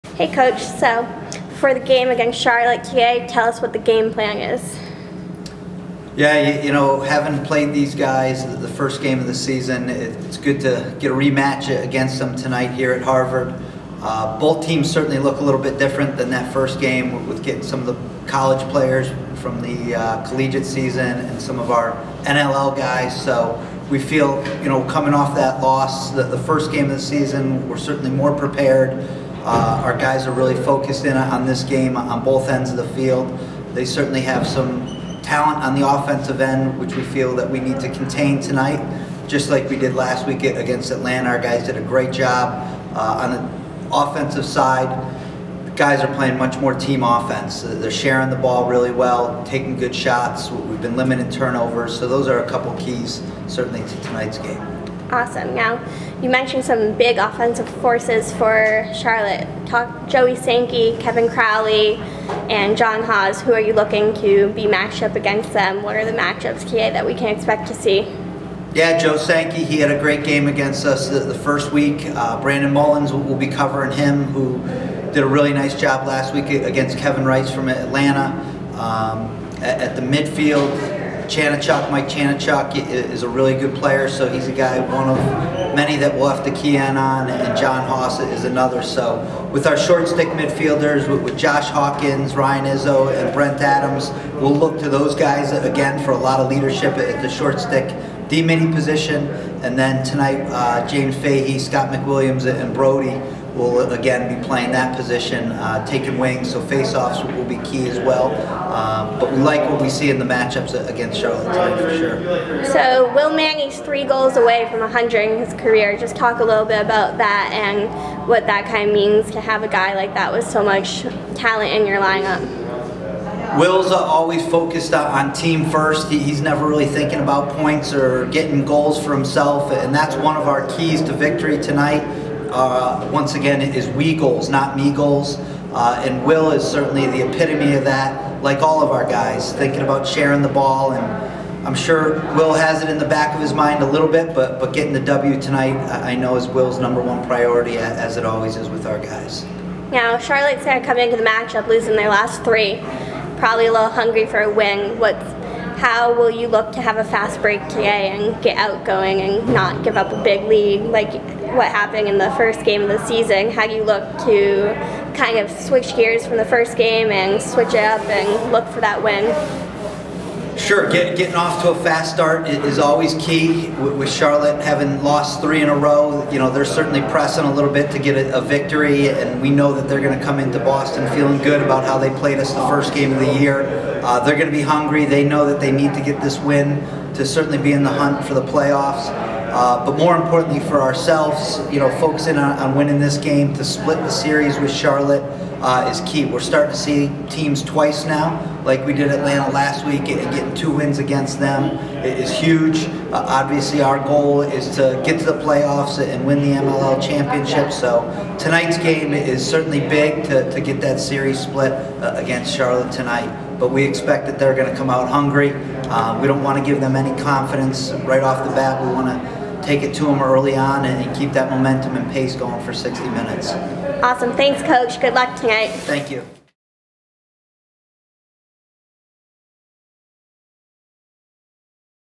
Kyocera New England Pregame Interview 6.11: Sean Quirk
Head Coach, Sean Quirk gives his thoughts in a pregame interview before tonight's rematch with the Charlotte Hounds at Harvard Stadium. Pregame interviews presented by Kyocera New England.